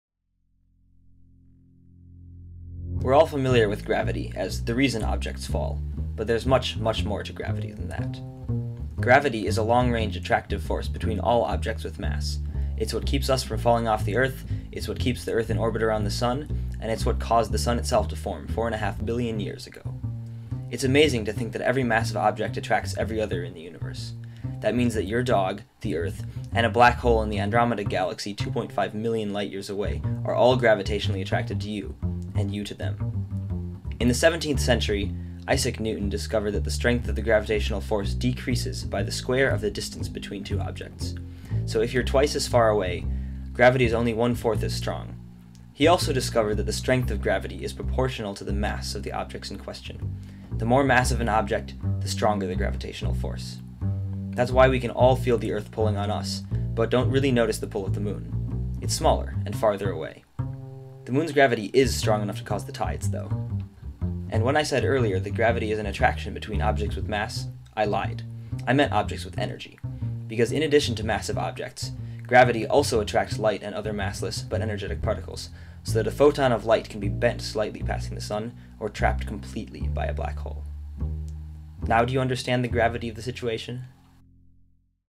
Minute Physics: What is Gravity?
One Minute Physics provides an energetic and entertaining view of old and new problems in physics -- all in one minute!

In this episode, we discuss the basic nature of gravity, one of the four fundamental forces in our universe.